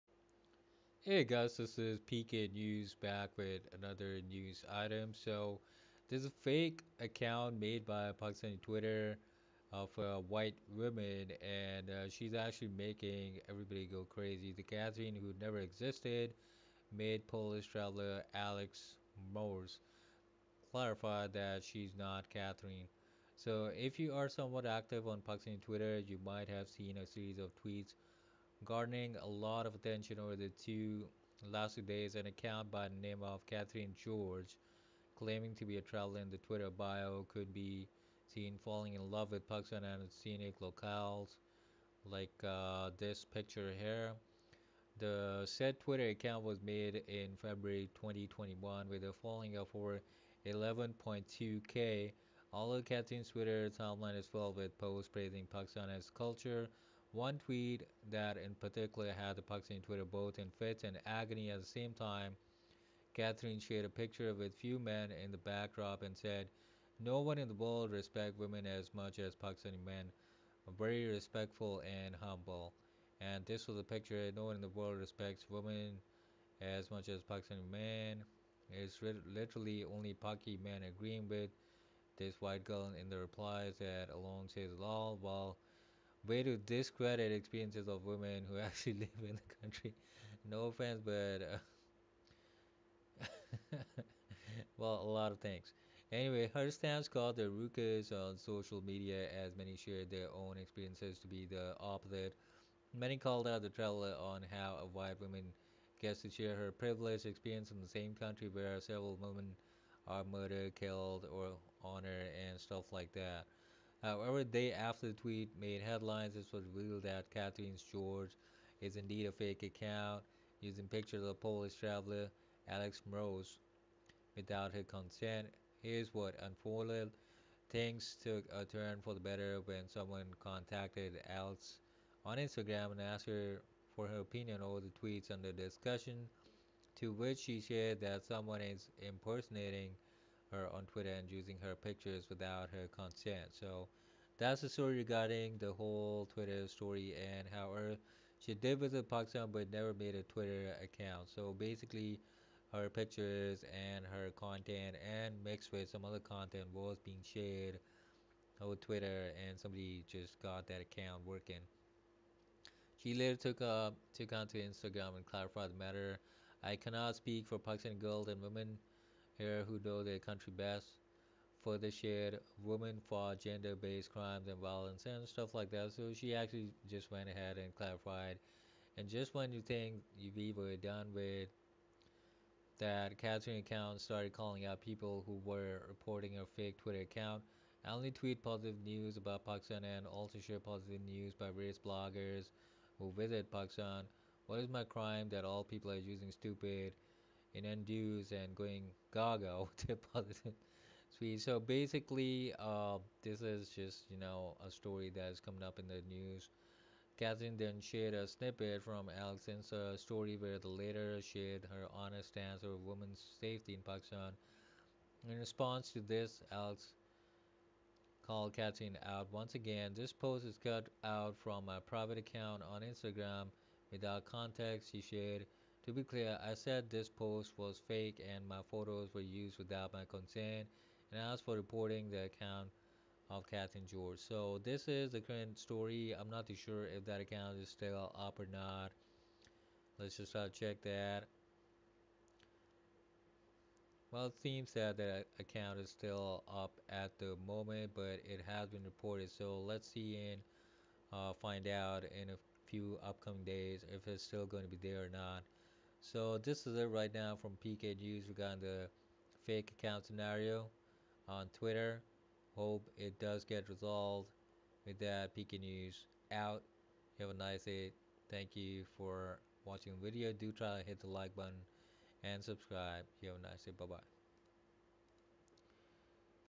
When a white woman’s fake account made Pakistani Twitter go crazy
The Katherine who never existed made Polish traveller Aleks Mroz clarify that she is not Katherine

If you are somewhat active on Pakistani Twitter, you might have seen a series of tweets garnering a lot of attention over the last two days. An account by the name of Katherine George, claiming to be a traveler in the Twitter bio, could be seen falling in love with Pakistan and its scenic locales.